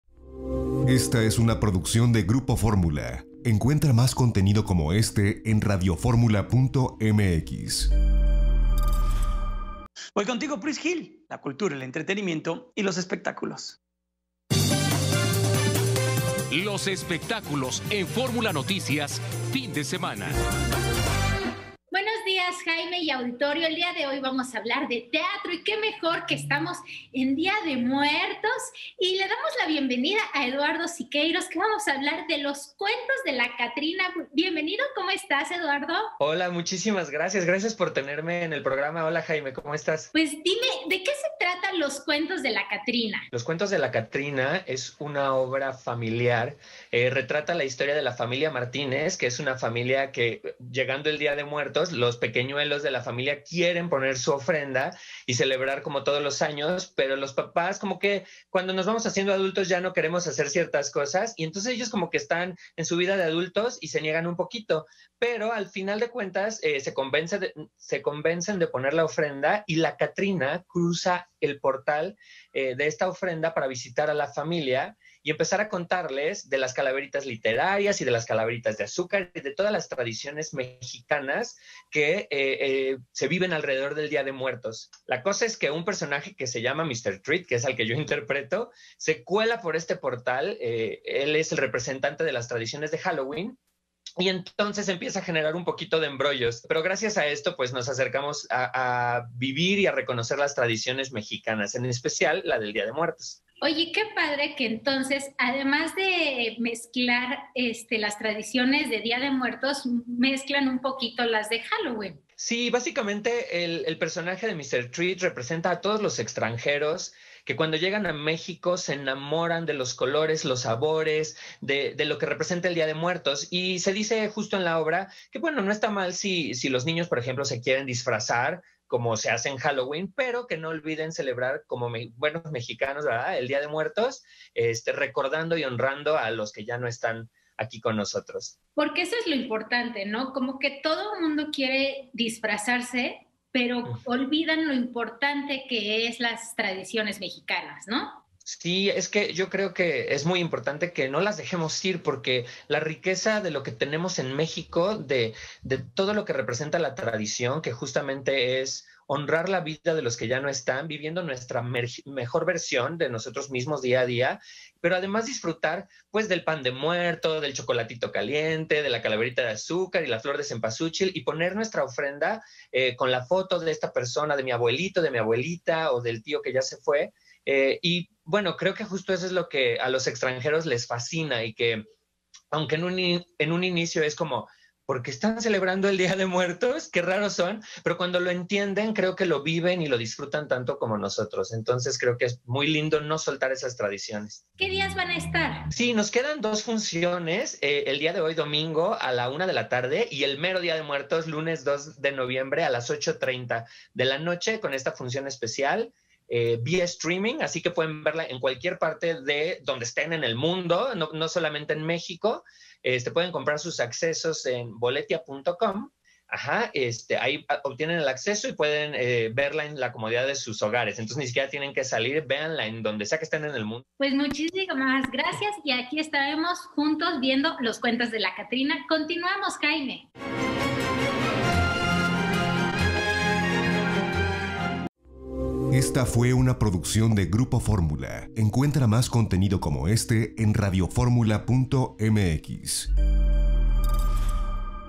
Los espectáculos con Pryz Hill 011120
Los espectáculos con Pryz Hill
 nos habla de @CuentosCatrina
 no te pierdas las ultimas funciones